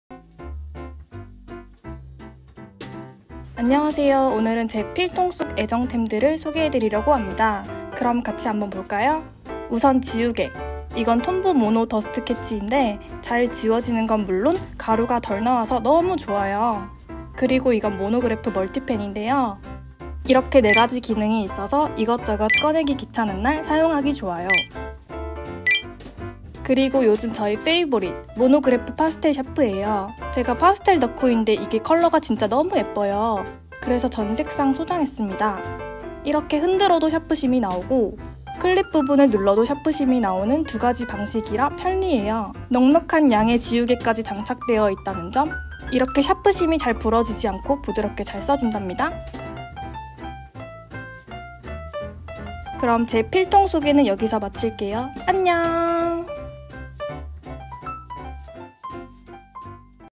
톰보│What's in my pencil case
What’s in my pecncil case?

톰의 필통 속 같이 구경하자!
톰의 최애 샤프도 공개 (속닥속닥)